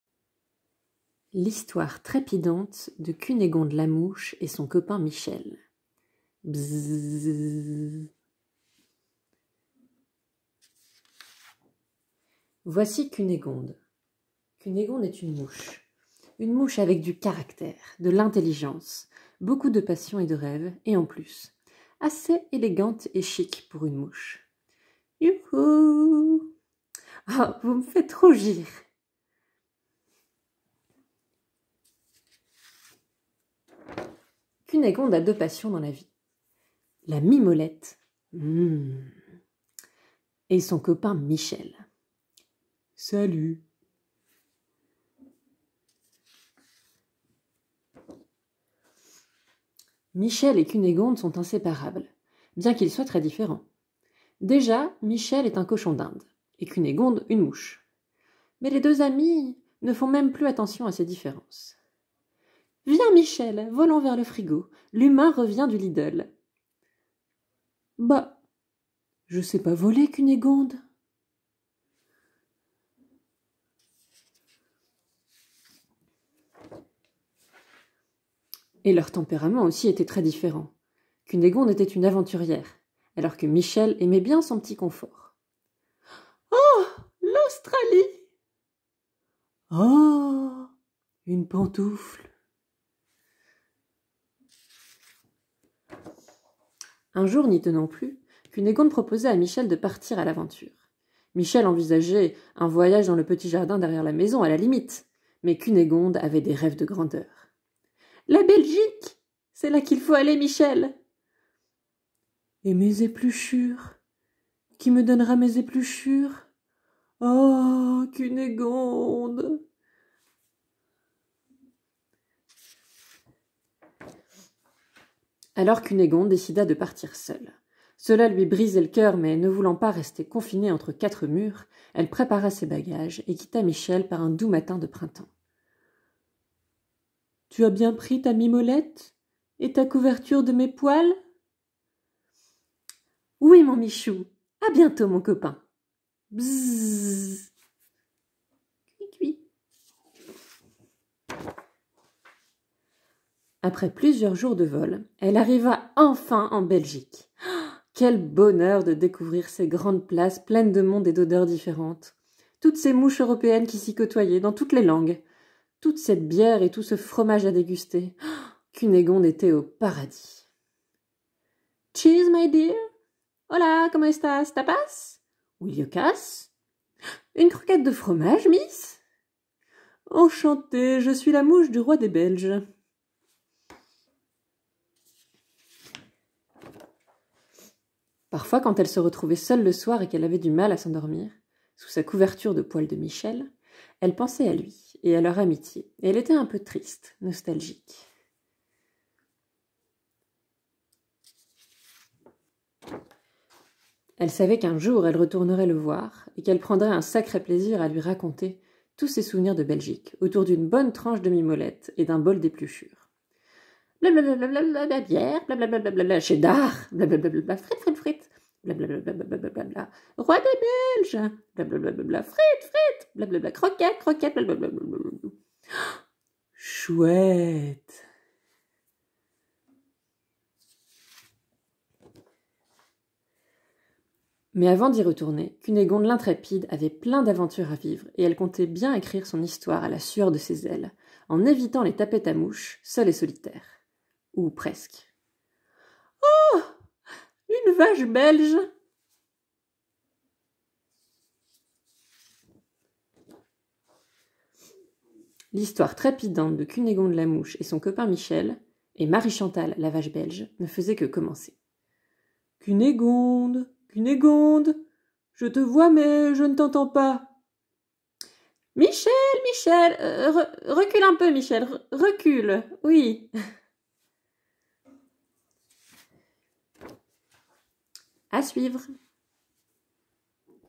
L'histoire trépidante de Cunégonde la mouche et son copain Michel
écrit, dessiné et lu par Camille Vroman, bibliothécaire à Louise Michel :)